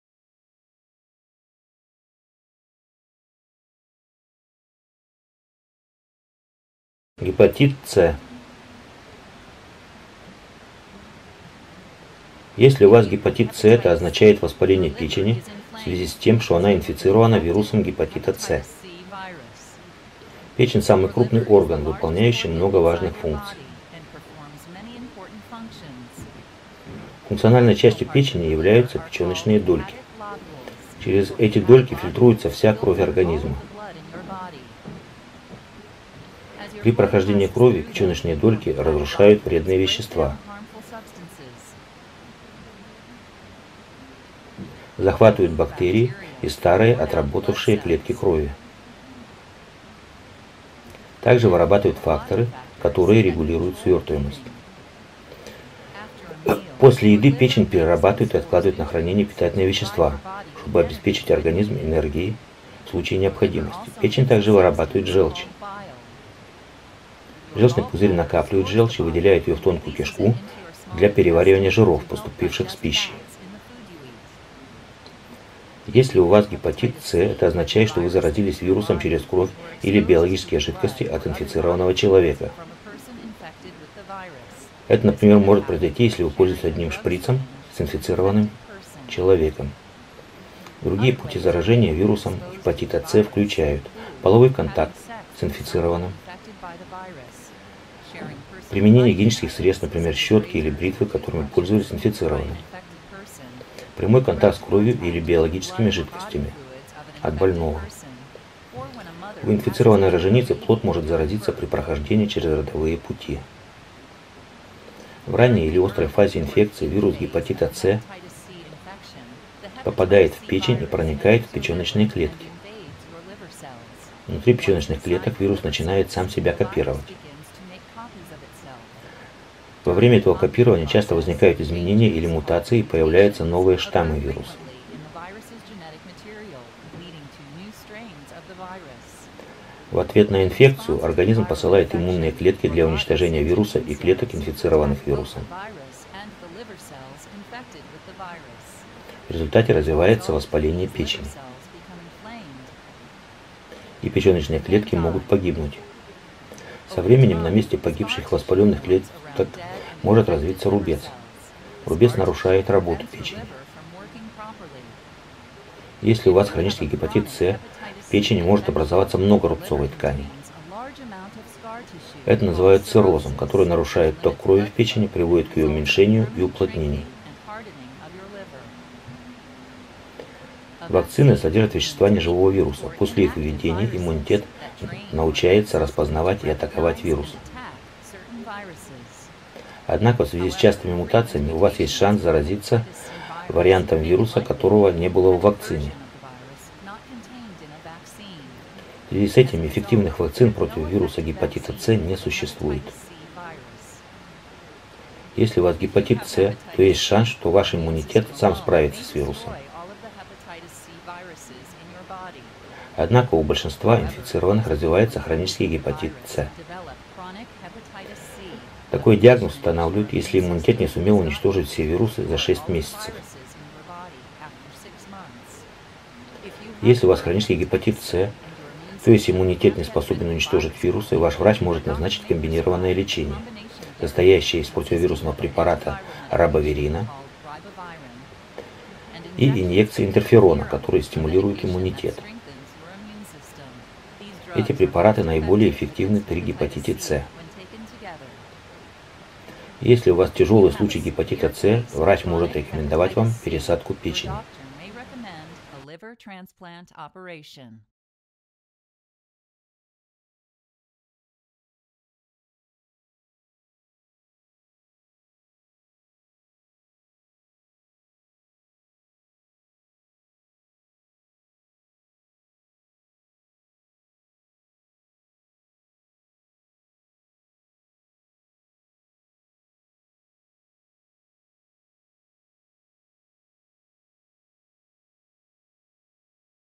Гепатит С
Павлодарский областной диагностический центр подготовил виртуальную школу для пациентов с болезнями печени, с которой можно познакомиться оп ссылке